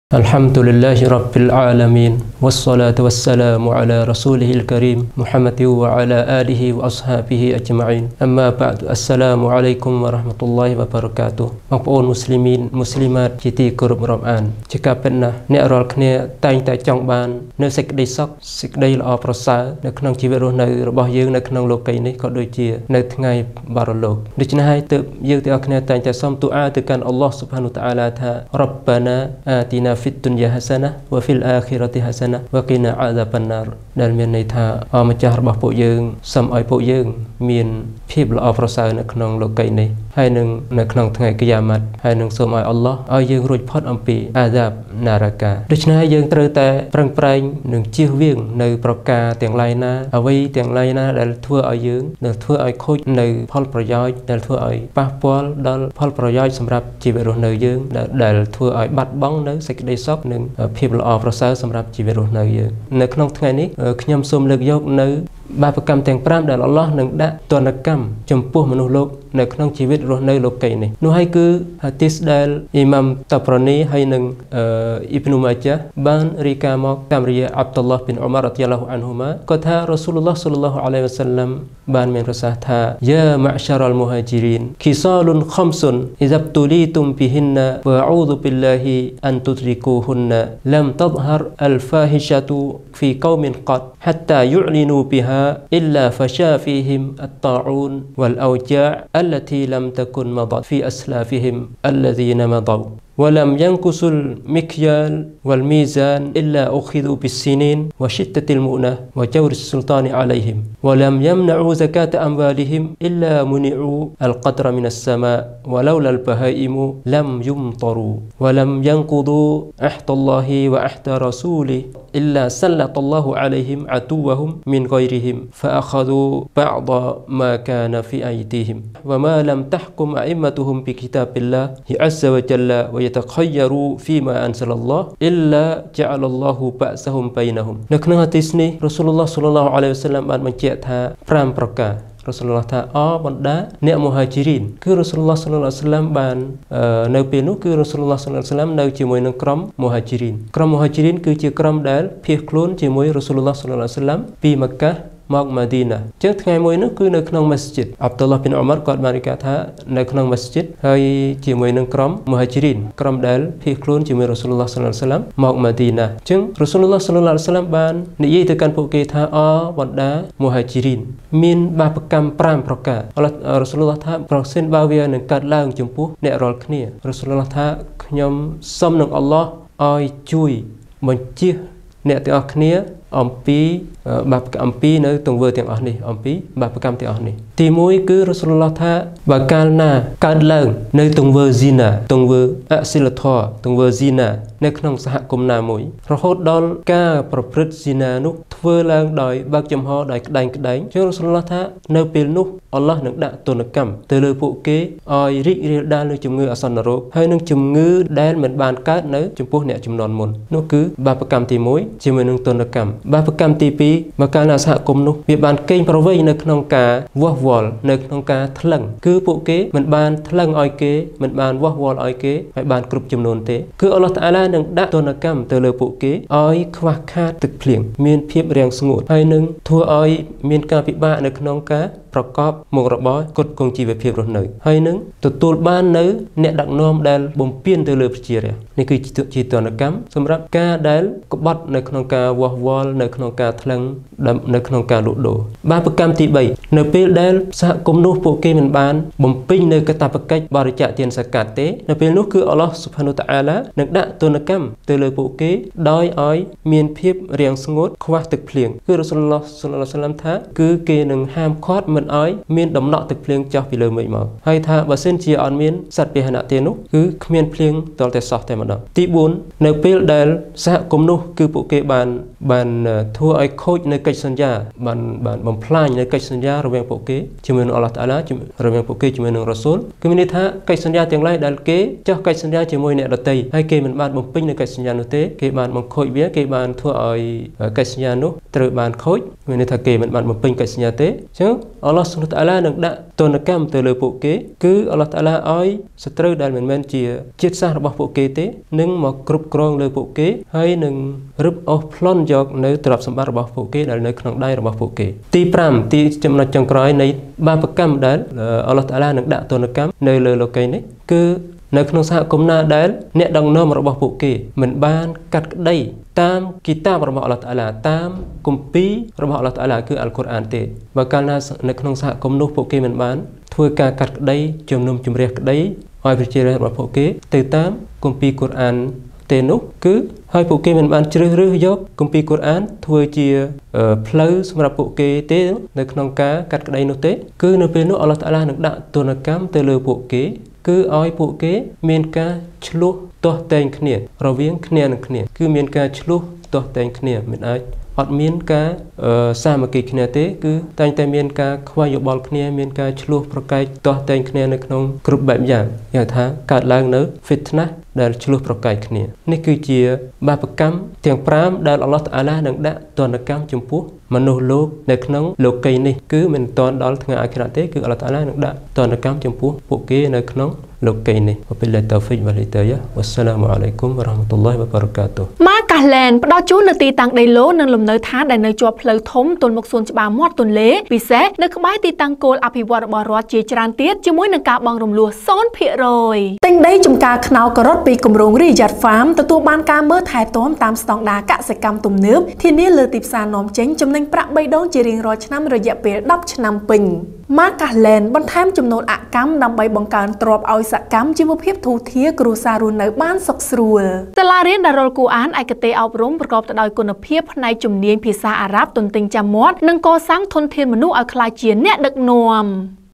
បាបកម្មទាំងប្រាំដែលអល់ឡោះនឹងដាក់ទណ្ឌកម្មក្នុងជីវិតរស់នៅលោកិយ
បកស្រាយដោយ៖ អូស្តើស ស្លៃម៉ាន អះម៉ាត់, អ្នកឯកទេសអប់រំក្នុងសាសនាឥស្លាម